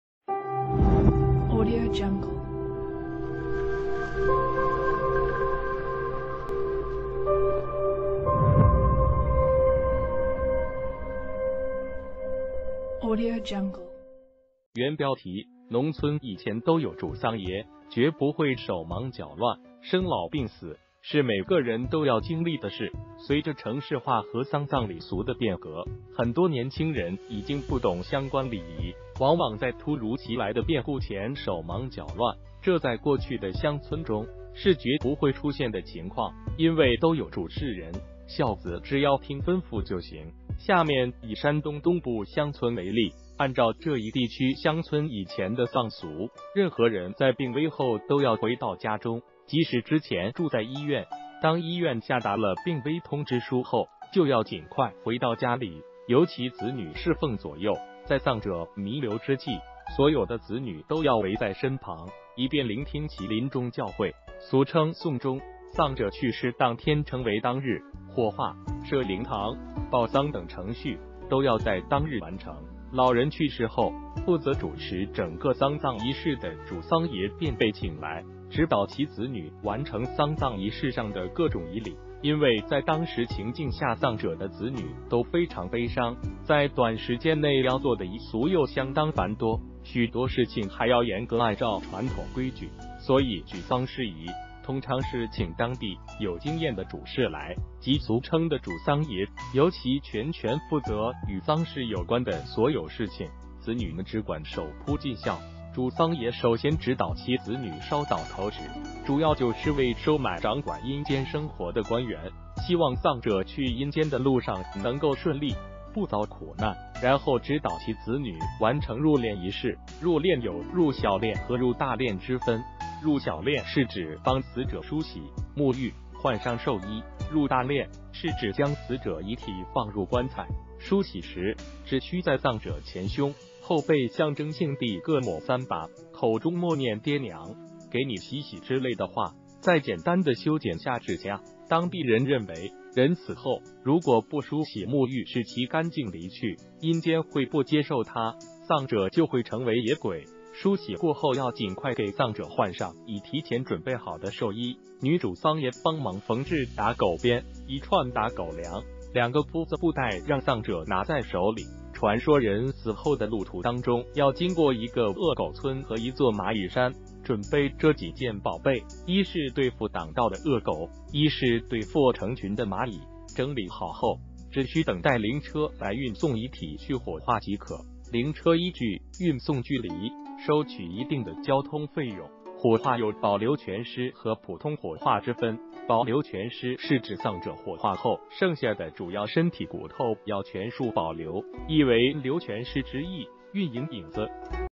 农村以前都有“主丧爷”，绝不会手忙脚乱！
原标题：农村以前都有“主丧爷”，绝不会手忙脚乱！生老病死，是每个人都要经历的事。随着城市化和丧葬礼俗的变革，很多年轻人已经不懂相关礼仪，往往在突如其来的变故前手忙脚乱。这在过去的乡村中，是绝不会出现的情况，因为都有主事人，孝子只要听吩咐就行。下面以山东东部乡村为例。按照这一地区乡村以前的丧俗，任何人在病危后都要回到家中，即使之前住在医院，当医院下达了病危通知书后，就要尽快回到家里由其子女侍奉左右。在丧者弥留之际，所有的子女都要围在身旁，以便聆听其临终教诲，俗称“送终”。丧者去世当天称为“当日”，火化、设灵堂、报丧等程序都要在“当日”完成。老人去世后，负责主持整个丧葬仪式的“主丧爷”便被请来，指导其子女完成丧葬仪式上的各种仪礼。因为在当时情境下丧者的子女都非常悲伤，在短时间内要做的仪俗又相当繁多，许多事情还要严格按照传统规矩，所以举丧事宜通常是请当地有经验的主事来，即俗称的“主丧爷”，由其全权负责与丧事有关的所有事情，子女们只管守铺尽孝。主丧爷首先指导其子女烧“倒头纸”，主要就是为“收买”掌管阴间生活的官员，希望丧者去阴间的路上能够顺利、不遭苦难，然后指导其子女完成入殓仪式。入殓有“入小殓”和“入大殓”之分，“入小殓”是指帮死者梳洗、沐浴、换上寿衣；“入大殓”是指将死者遗体放入棺材。梳洗时，只需在丧者前胸、后背象征性地各抹三把，口中默念“爹（娘），给你洗洗”之类的话，再简单地修剪下指甲。当地人认为，人死后，如果不梳洗沐浴使其干净离去，阴间会不接受他，丧者就会成为野鬼。梳洗过后要尽快给丧者换上已提前准备好的寿衣，女主丧爷帮忙缝制“打狗鞭”、一串“打狗粮”、两个“麸子”布袋让丧者拿在手里。传说人死后的路途当中要经过一个“恶狗村”和一座“蚂蚁山”，准备这几件宝贝，一是对付挡道的恶狗，一是对付成群的蚂蚁。整理好后，只需等待灵车来运送遗体去火化即可，灵车依据运送距离收取一定的交通费用。火化有“保留全尸”和普通火化之分。“保留全尸”是指丧者火化后剩下的主要身体骨头要全数保留，意为“留全尸”之意。运营/影子